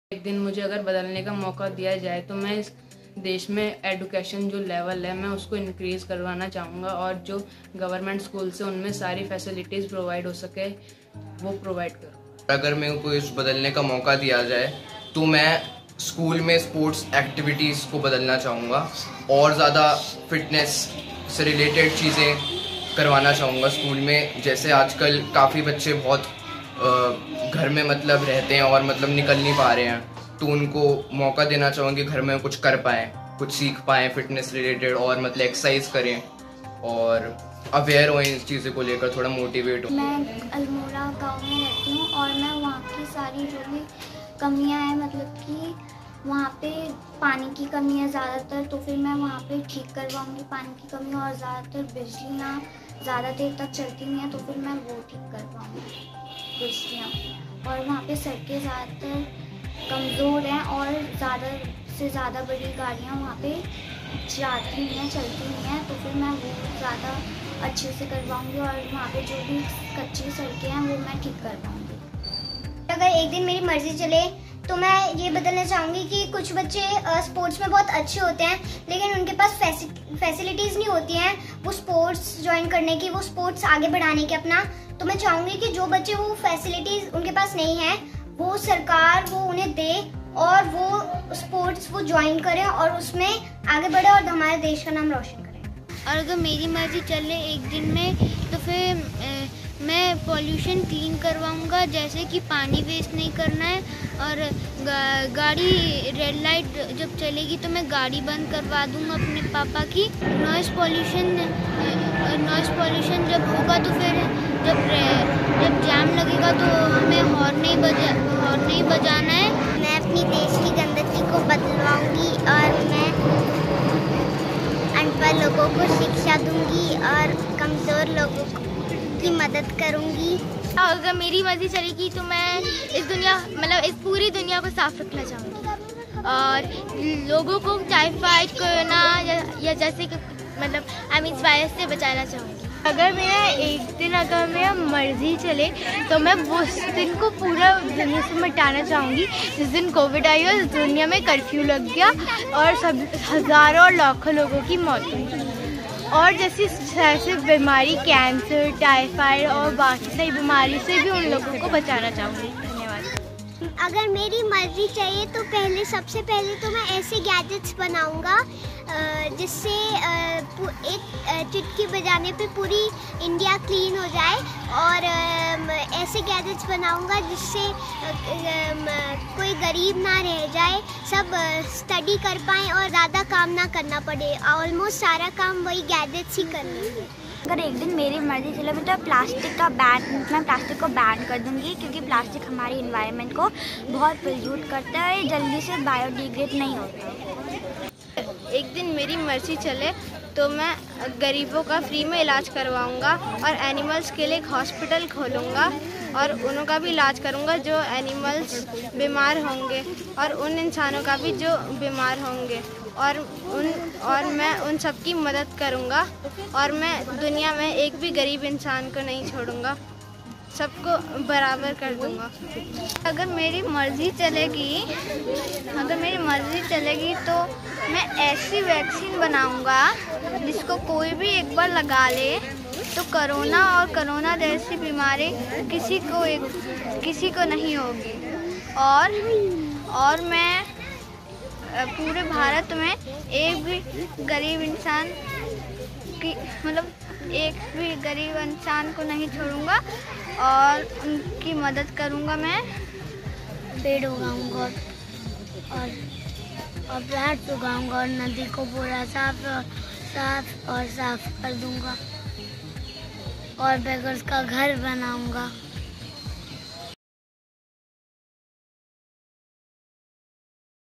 अगर एक दिन के नायक बने तो क्या करेंगे ? सुनिए इन बच्चों के मन की बात | ETV Bharat Jharkhand
देश आज आज़ादी की 75वीं वर्षगांठ मना रहा है. बच्चे, युवा, बुजुर्ग और महिलाएं सभी इस जश्न में शामिल हैं. आज हम आपको कुछ बच्चों के मन की बात सुना रहे हैं. इन बच्चों की बातों को सुनकर लग रहा है कि देश और दुनिया का भविष्य सुनहरा है. इनसे हमने पूछा कि अगर आपको एक दिन की कमान सौंप दी जाए तो ये अपने लिए क्या करेंगे. अपने देश के लिए क्या करेंगे. पर्यावरण को कैसे मैनेज करेंगे. इन नन्हें-मुन्ने बच्चों के मन की बात को आप भी सुनिए.